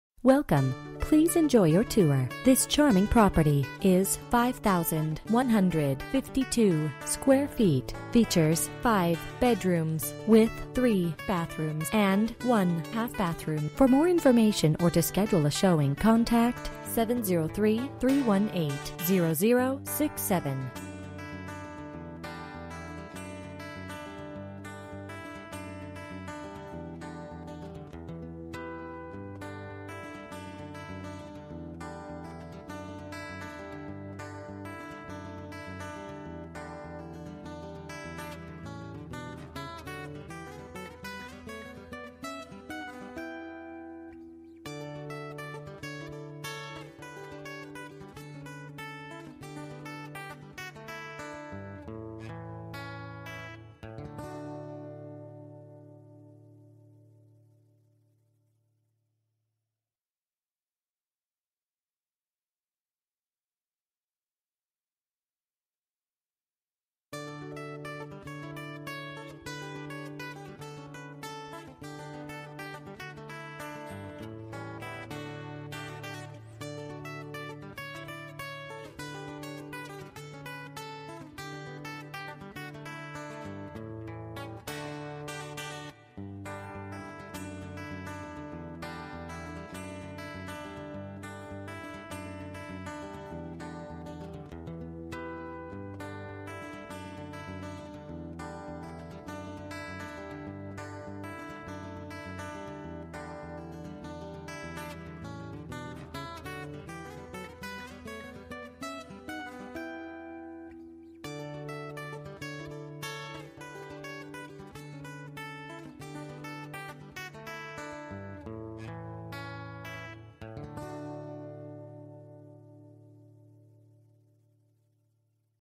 Residential for sale - 420 FALLS CHAPEL COURT, PURCELLVILLE,
Beautiful 5 bedroom 3.5 bath colonial located at the end of a cul-de-sac. 3 car garage, stunning deck overlooks manicured backyard, 2 story stone to ceiling fp, large cherry kitchen cabinets, huge office, fully finished basement, spacious master bedroom w/sitting room and luxury bath. A must see!

Bedrooms: 5
Bathrooms: 3 full and 1 half bathrooms
Square Feet: 5152
Price: $609,900

MLS ID: LO8609816

Array05/31/2015 08:44:02 am

Last modified: